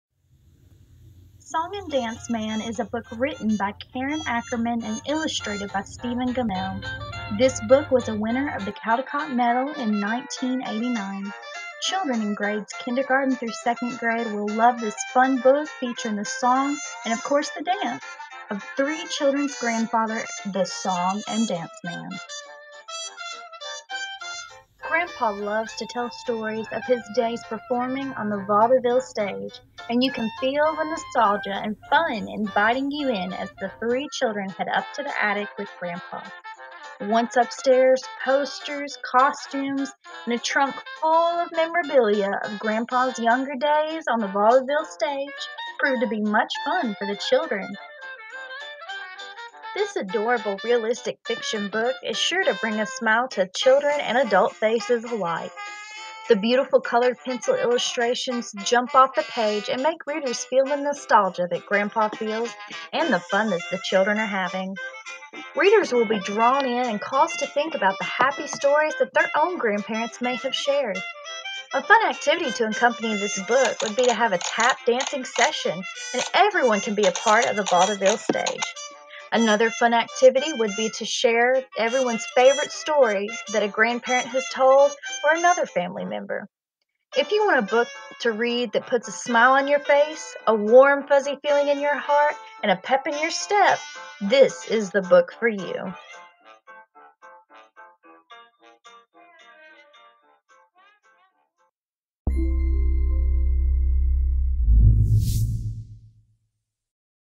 Song and Dance Man- INSC571 Book-Trailer
Book trailer on Song and Dance Man by Karen Ackerman and illustrated by Stephen Gammell. Created by Savannah A Gilbert. Created for INSC571 for Spring 2018 semester.

Ackerman, K., & Gammell, S. (1999). Song and Dance Man. New York: Knopf.
Target Audience: Kindergarten – Second Grade / Ages 5-8
Song and Dance Man is a book written by Karen Ackerman and illustrated by Stephen Gammell. This book was a winner of the Caldecott Medal in 1989. Children in grades kindergarten through second grade will love this fun book featuring the song and of course dance of three children’s grandfather, the song and dance man. Grandpa loves to tell stories of his days performing on the vaudeville stage and you can feel the nostalgia and fun inviting you in as the three children head up to the attic with grandpa. Once upstairs, posters, costumes and a trunk containing memories of Grandpa’s younger days prove to be much fun and the children pretend they too are on the vaudeville stage. This adorable realistic fiction book is sure to bring a smile to children and adults faces alike. The beautiful colored pencil illustrations jump off the page and make you feel the nostalgia grandpa feels and the fun the children are having. Readers are drawn in and cause one to think about the happy stories their own grandparents may have shared.  A fun activity to accompany this book would be to have a tap dancing session and have everyone be a part of the vaudeville stage or have children share their favorite story about a grandparent or other family member! If you want a book to read that puts a smile on your face, a warm fuzzy feeling in your heart, and a pep in your step, this is the book for you.